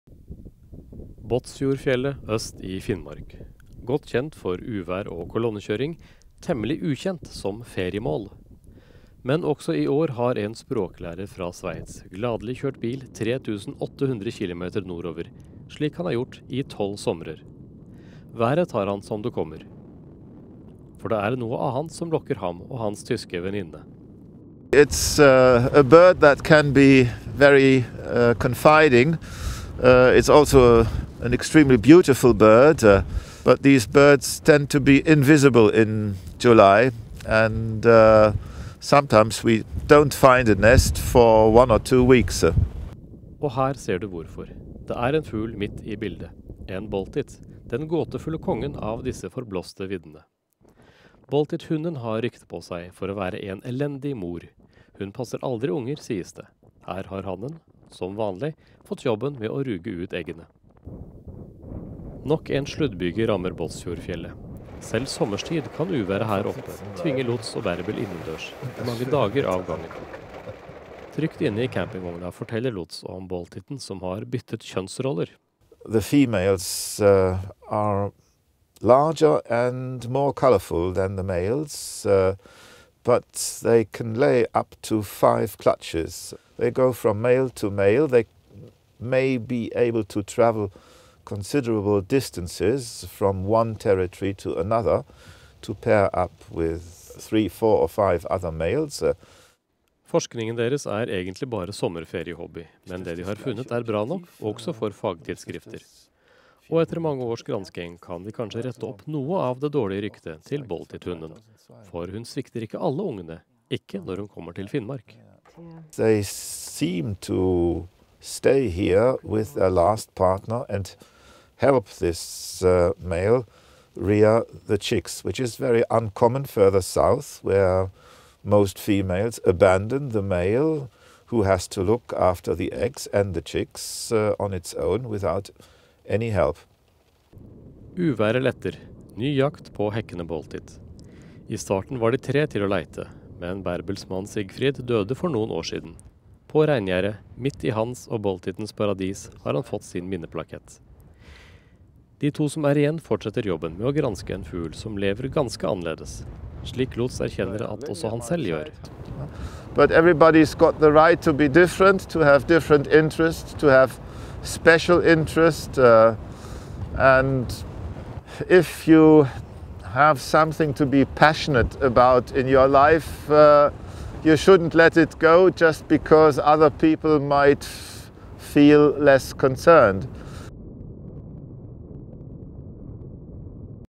Boltit Dotterel Varanger
Lutz Lücker on Dotterel (Boltit) research on the Varanger Peninsula, Båtsfjord, Norway.